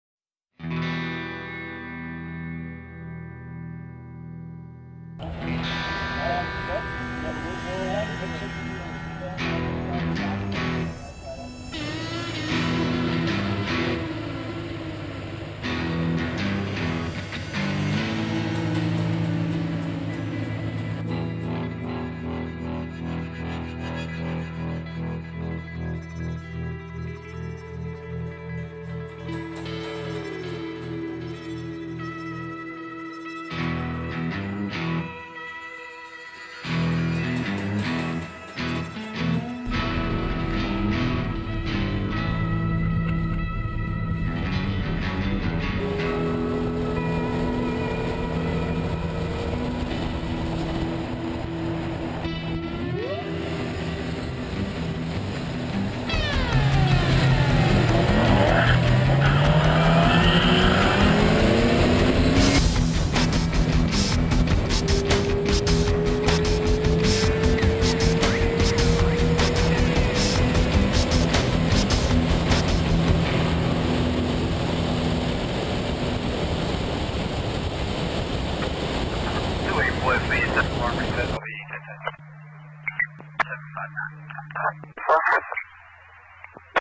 Nightmares on the 401
Me and some bros from the Kingston area Mustang club doing what we do :)